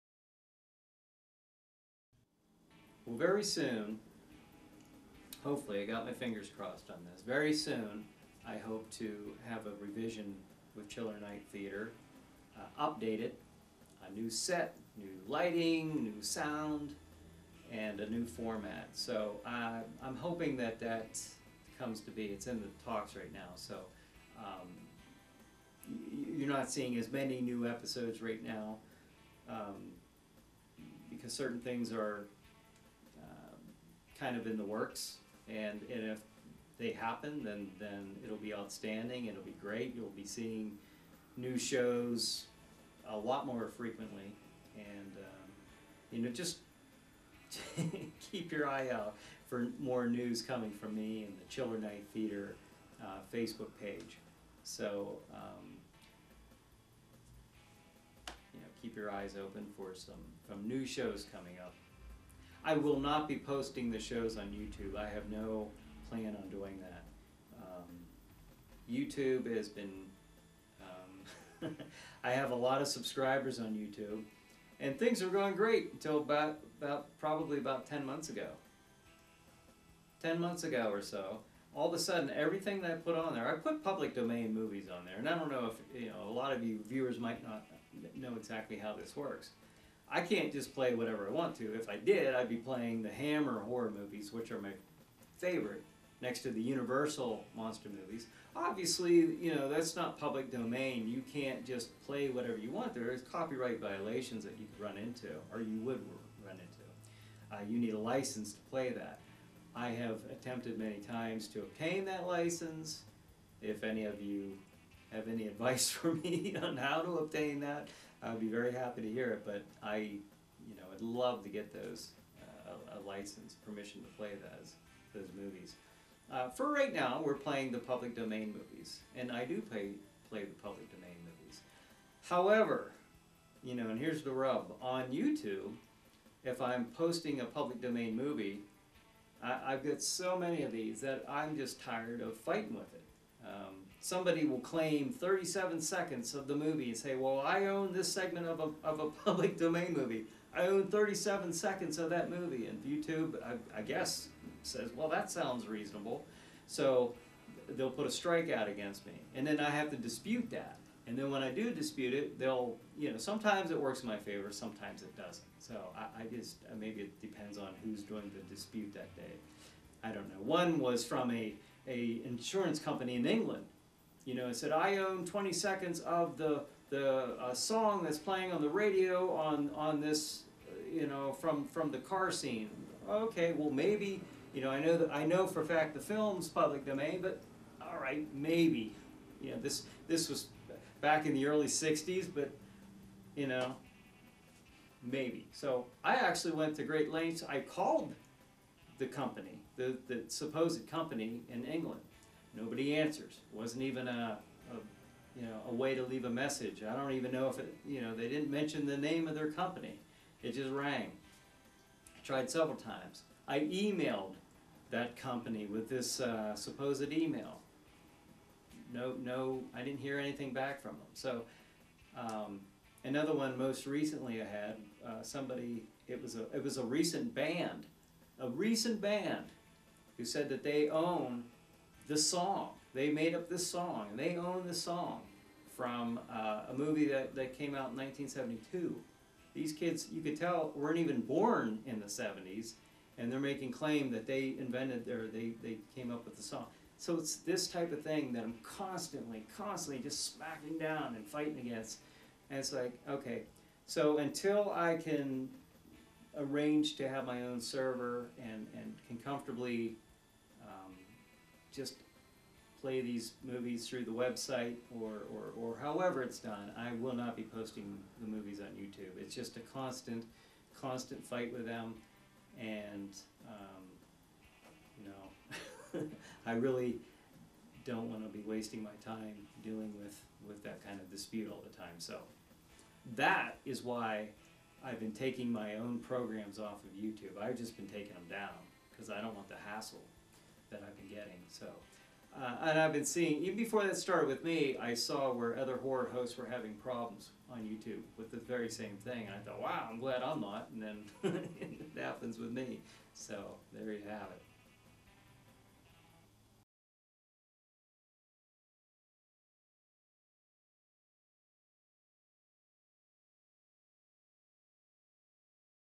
NEW episodes and why I will NOT be posting them on this channel anymore
NEW shows will ONLY be available to watch on TV.  Unfortunately I have been bombarded with fake copyright violation claims.  I will perhaps arrange to have the show available again online, but I will NOT be posting any more on YouTube!  Too much nonsense on here in the last 10 months.